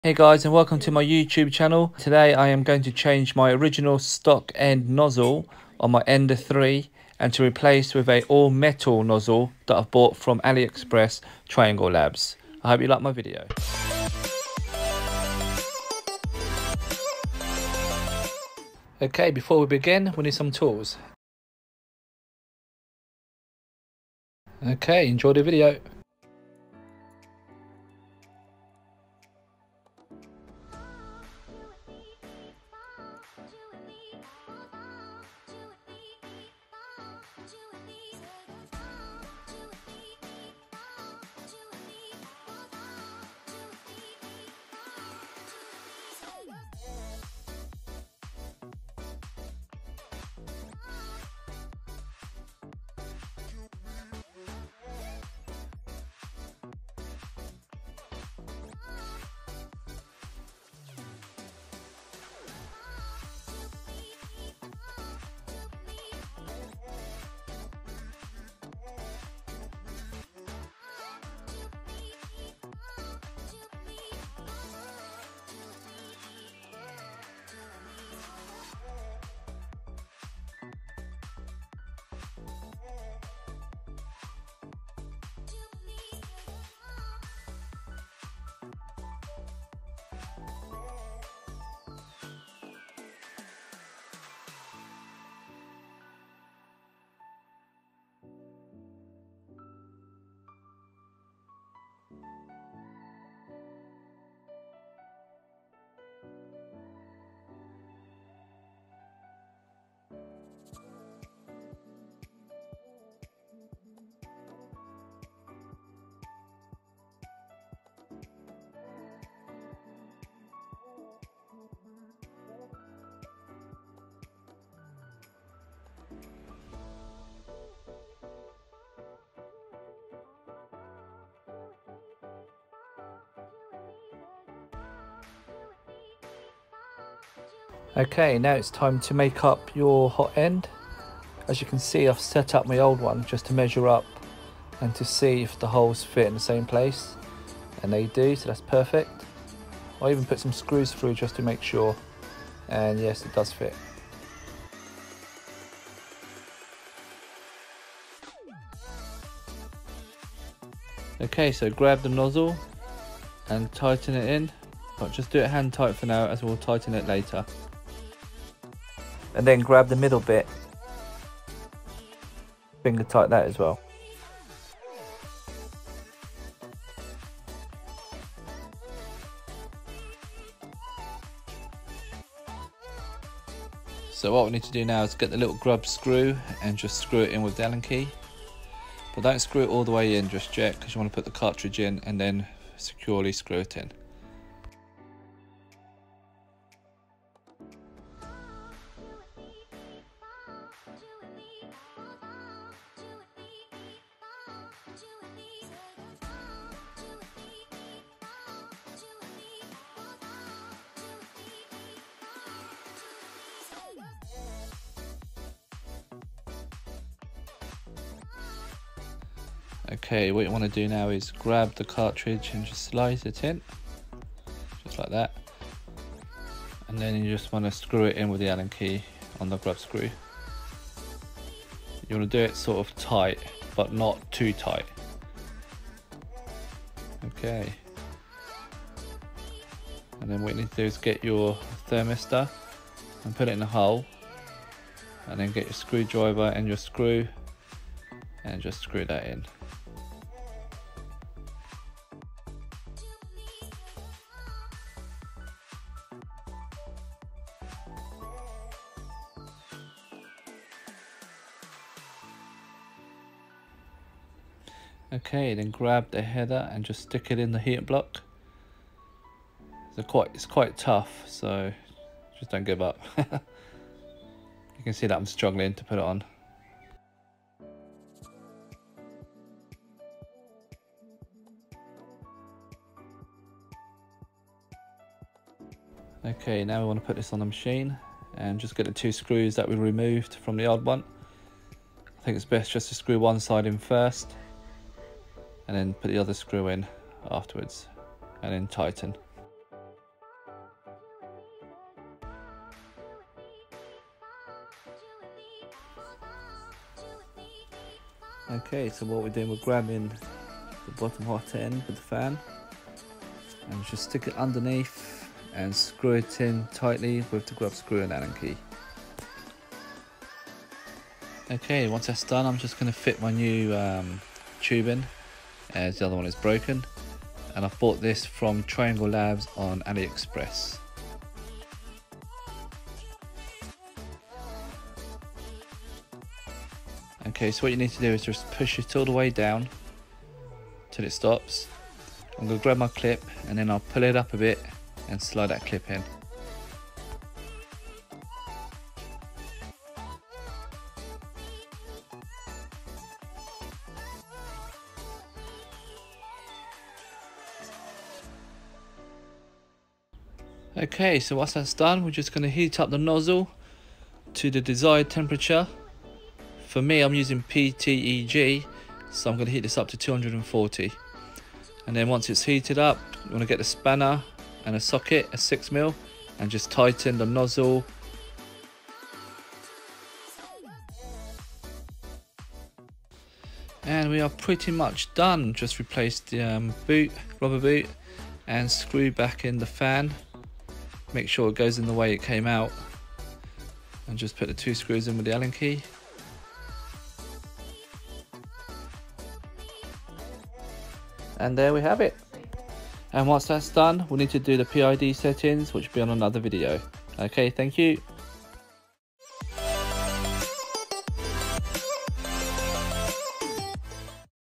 Upgrading Creality Ender 3 Stock Hotend Nozzle To AllMetal Hotend AliExpress Trianglelabs Swiss CR10
In this video you will see, that I am changing my stock end nozzle to an all metal nozzle as I would like to print in PETG and other materials.

My printer is creality Ender 3 , is this will also work for other example 3 pro cr 10 etc

My Ender 3 maximum temp is 240 anything above affects the Ptfe tubing and breaks down over time and releases toxic chemicals

I have never replaced this before so it’s my first time after watching videos and reading manuals

I bought the all metal from triangle labs on Ali express for about £23 and so far it’s working well.

Tools I’ve used are

Allen keys that came with the Ender 3
Spanner
6mm socket and wrench/ or spanner
7mm spanner

Once you have finished I’ve read that it’s best to calibrate the PID settings as the nozzle produces new temperatures  so I have made a video for this also which I would link to video here.

Join AliExpress for discounts

Hi, it's fun to shop on AliExpress! Tap to know more

Swiss CR10 all metal hit end

Hope you like video and maybe learn from it too